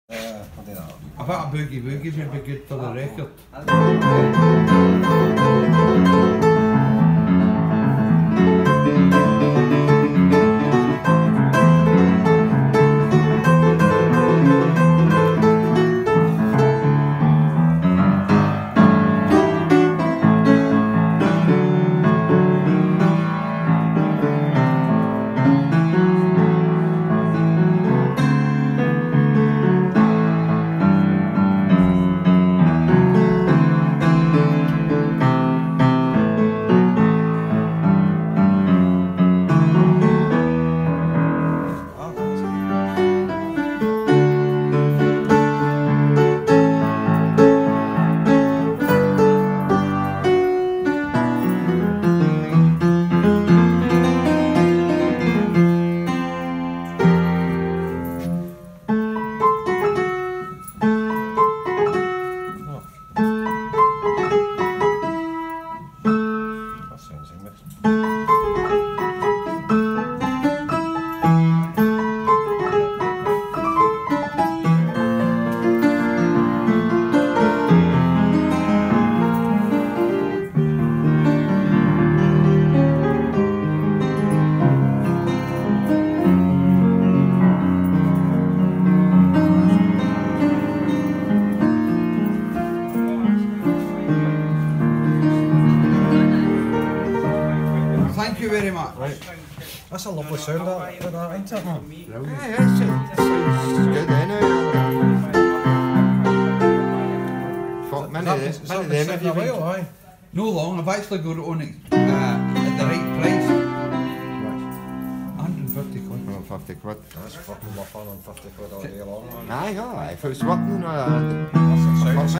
ERARD Upright Overstrung Pianoforte For Sale
Quality upright piano for sale .
Can be viewed at the ARTI SAN Toi Gallery
In the heart of the Glasgow Barras .
Please send a message if you would like to view .